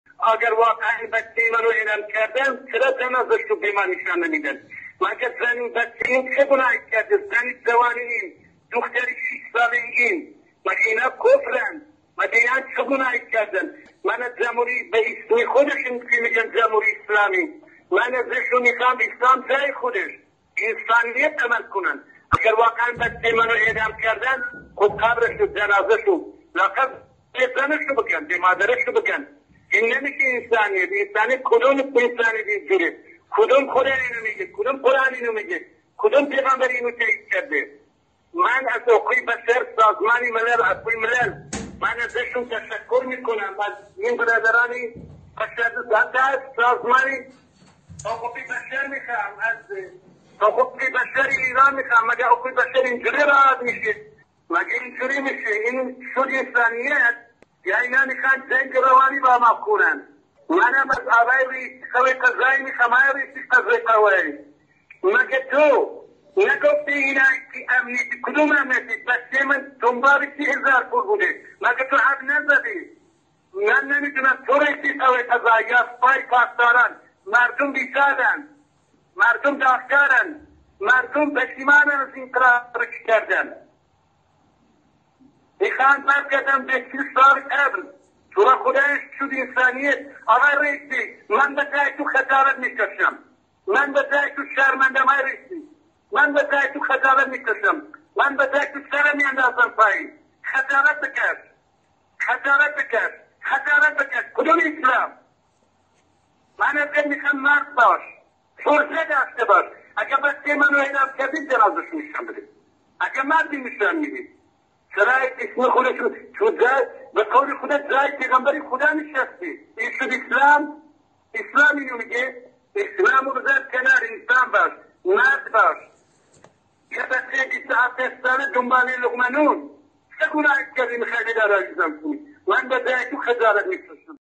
دادخواهی پدر هدایت
دادخواهی پدر هدایت عبدالله پور برای اعدام فرزندش